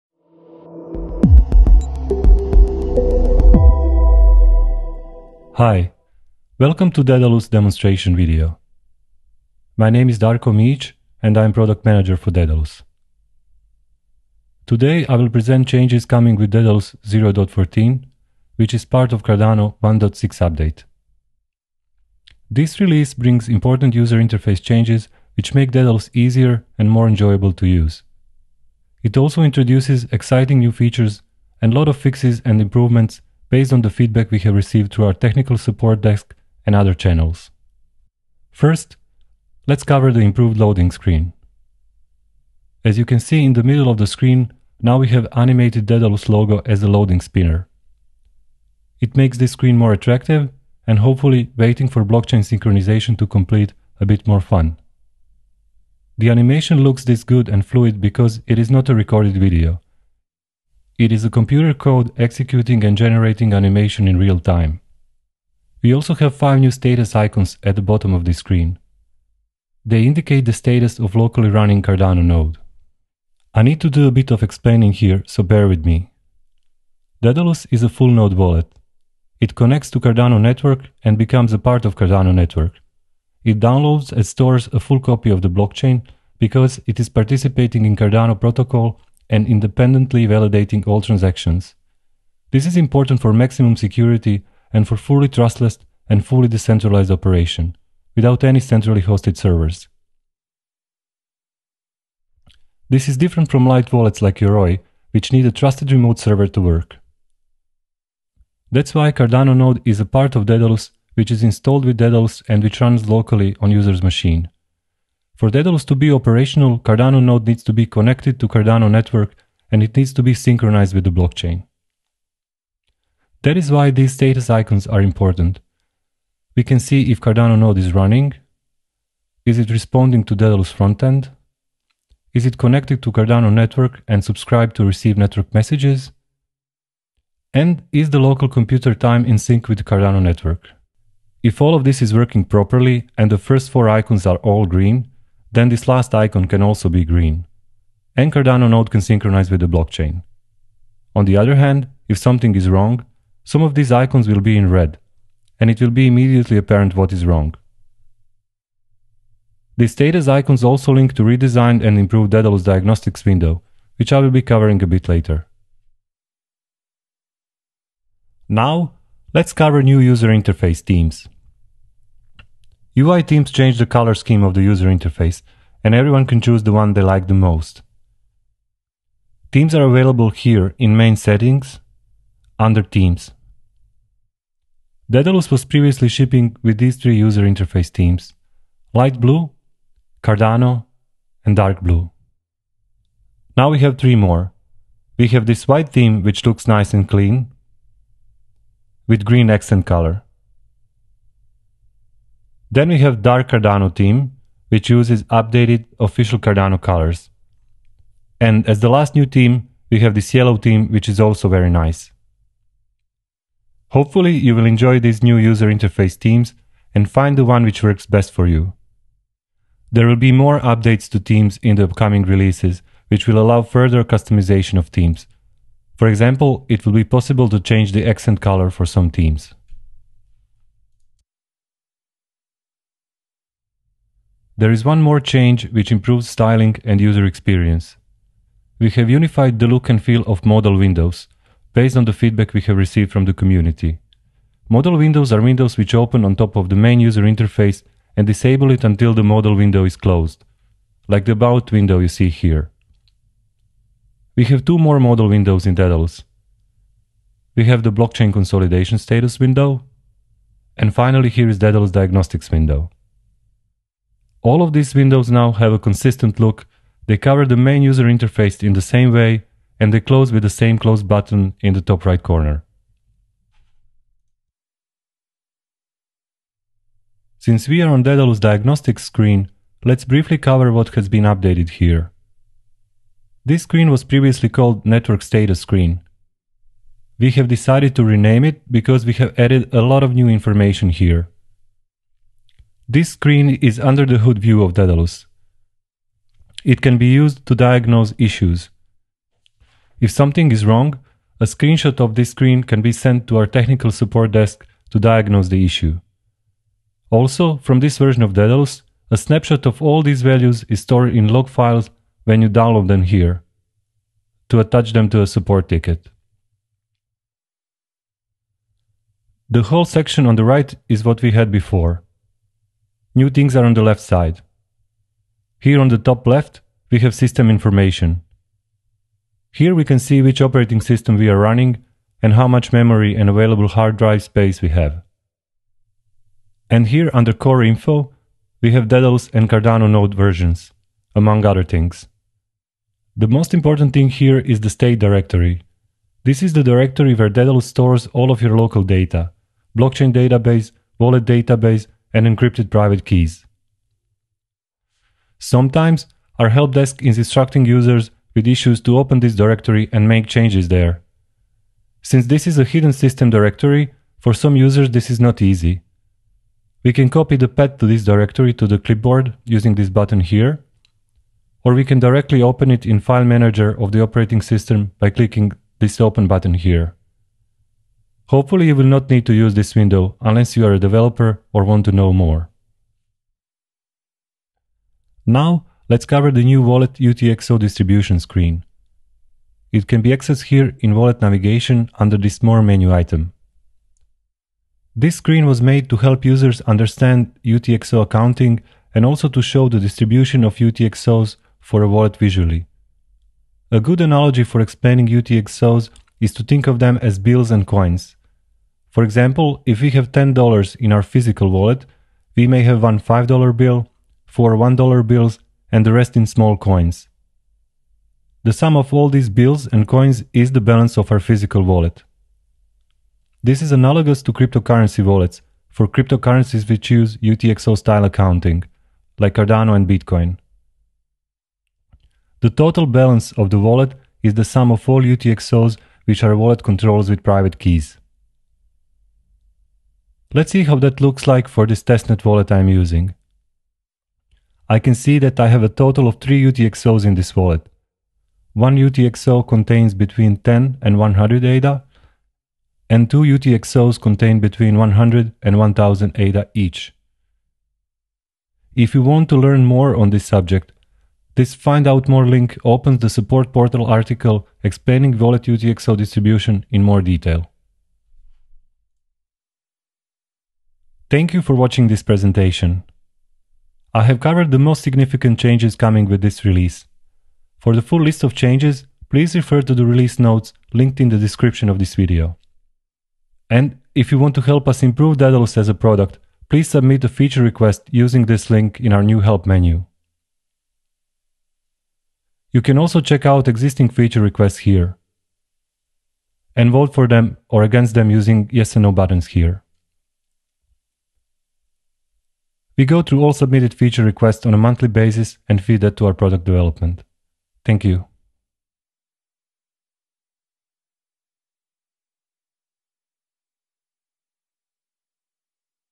OUT NOW: New Daedalus 0.14.0 with updated features and enhancements
Here's an introduction to the features you'll find in the new Daedalus wallet 0.14.0, which is part of Cardano 1.6 release, presented by Darko Mijić, Daedalus product manager.

This release brings a number of significant updates and improvements to the wallet experience, making it easier and more enjoyable to use, redesigned loading screens with status icons, new UI themes and Wallet UTXO distribution screen. It also adds a number of useful improvements to the support functions for faster diagnostics and issue resolution.